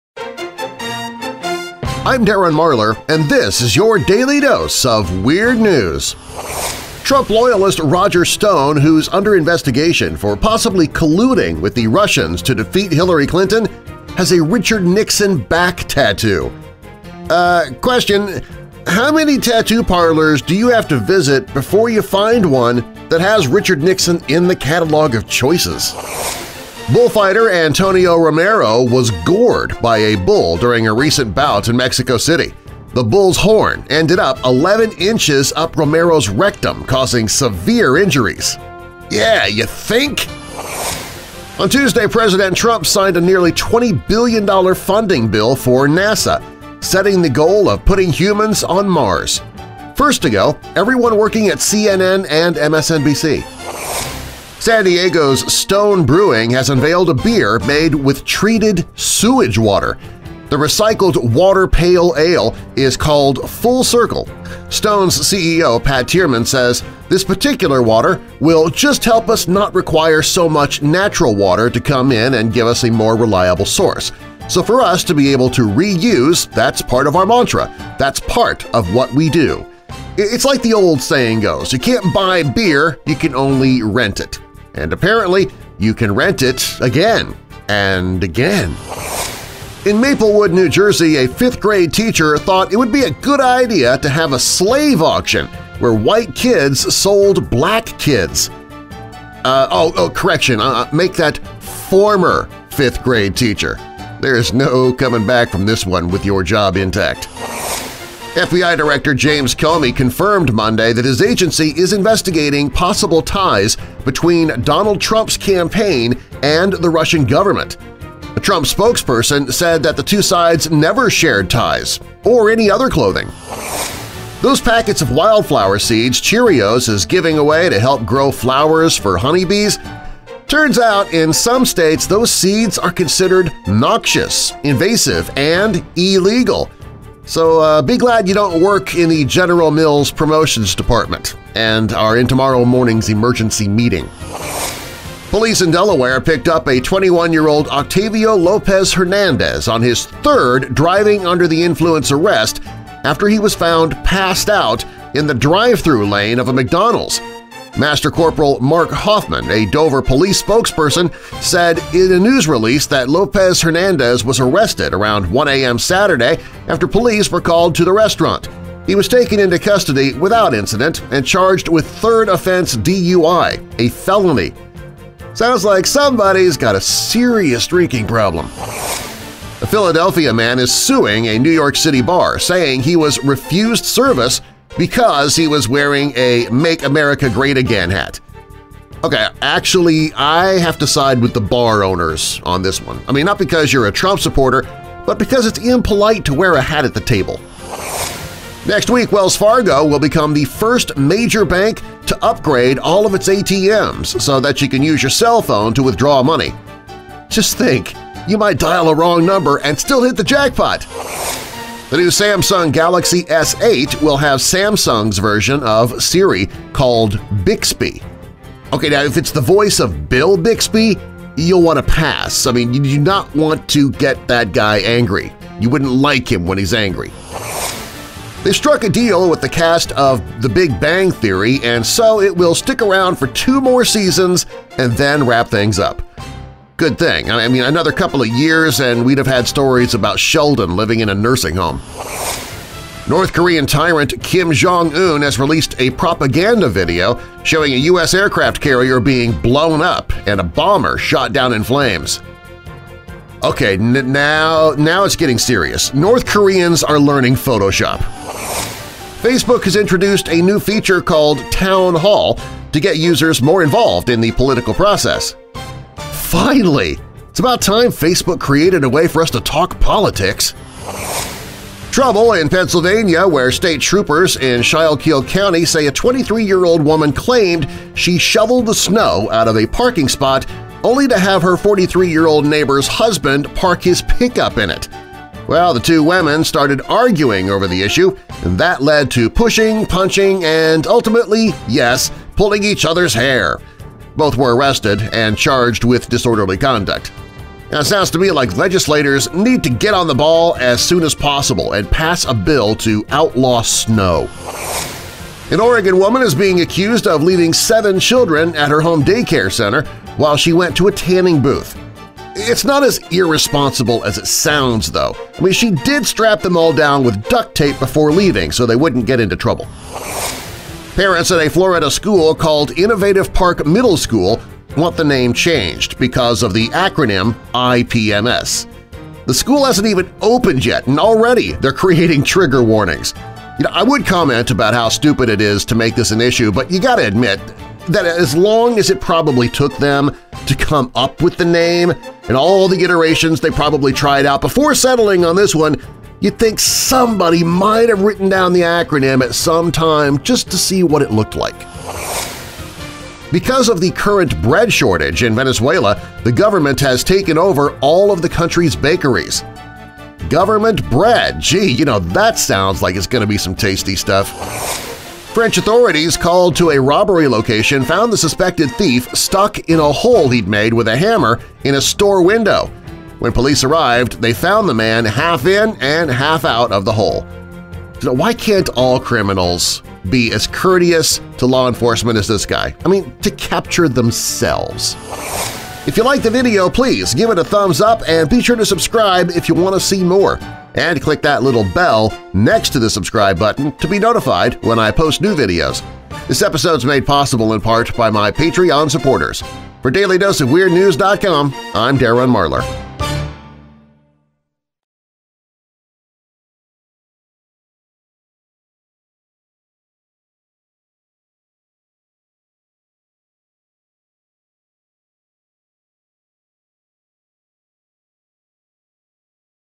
Would you drink sewage water beer? And 18 more weird news stories!
Bullfighter gored! * Sewage water beer! * Cheerios distributing illegal contraband! * Facebook promotes politics! * Woman leaves kids alone so she can get a tan! * Window captures thief! * Government takes over all bakeries! * And MORE in this DAILY DOSE OF WEIRD NEWS!

Thanks to my Weird Patrons: Maureen Anne MESI and Keith Conrad!